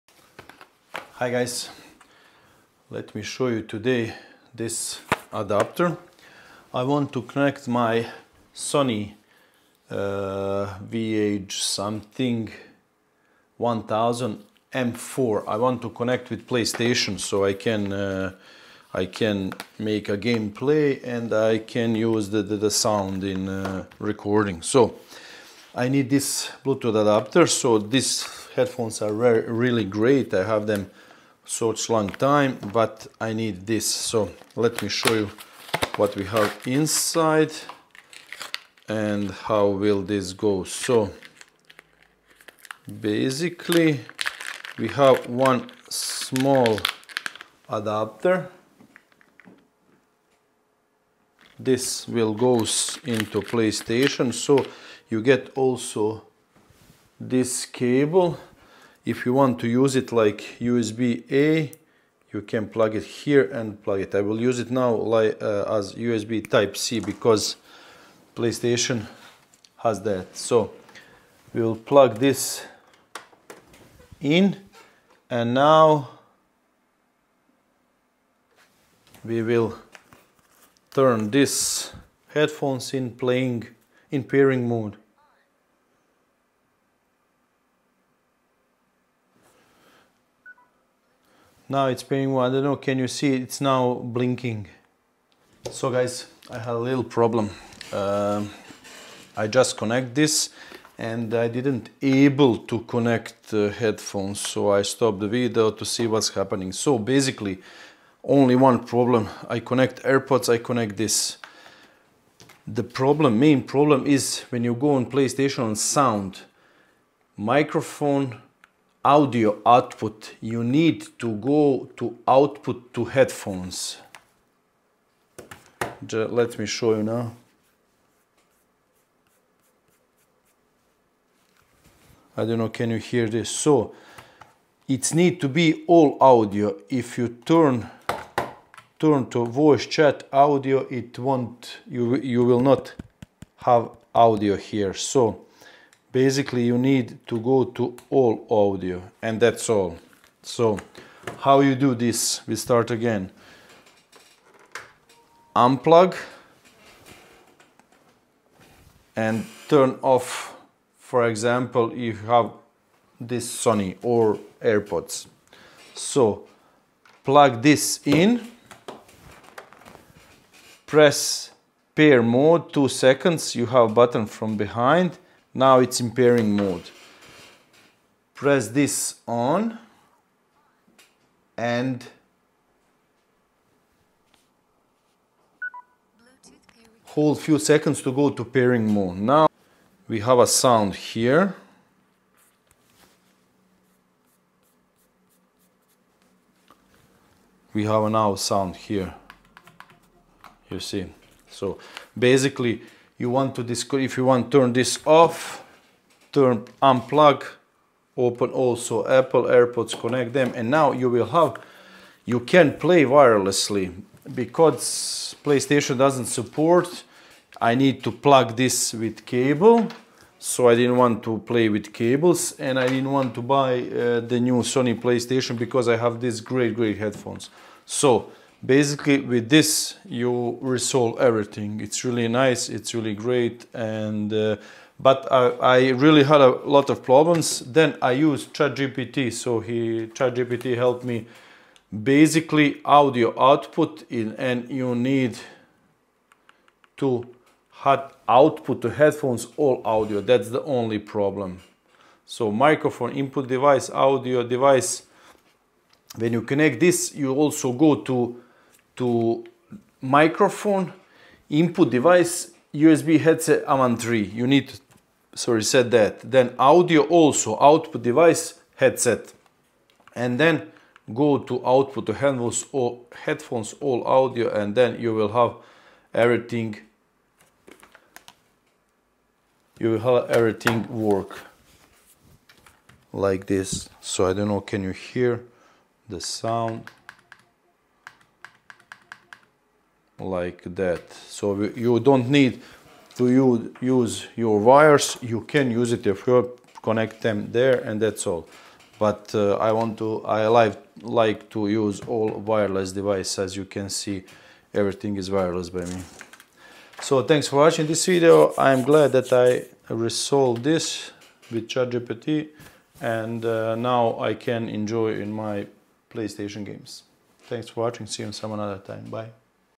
PS5 No Bluetooth Audio? Here’s the Fix with Avantree C81!
Are you struggling to connect Bluetooth headphones to your PS5? Sony doesn’t support Bluetooth audio natively, but with the Avantree C81, you can finally enjoy wireless audio on your PlayStation 5! In this video, I’ll show you how to set up Avantree C81, test its performance, and see if it’s the best solution for PS5 Bluetooth audio problems.